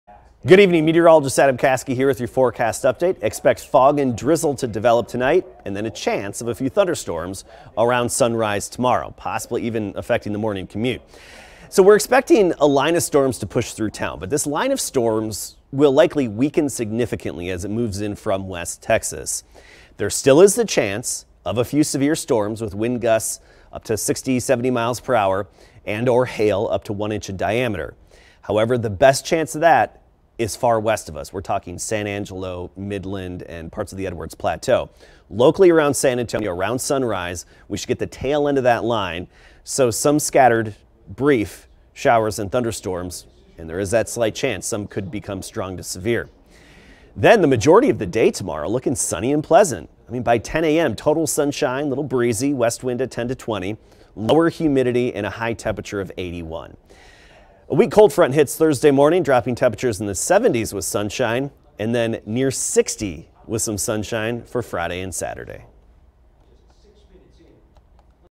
ADAM'S 5 PM UPDATE: MARCH 12TH
Expect fog and drizzle to develop tonight. There is also a chance of thunderstorms to pass through our area around sunrise.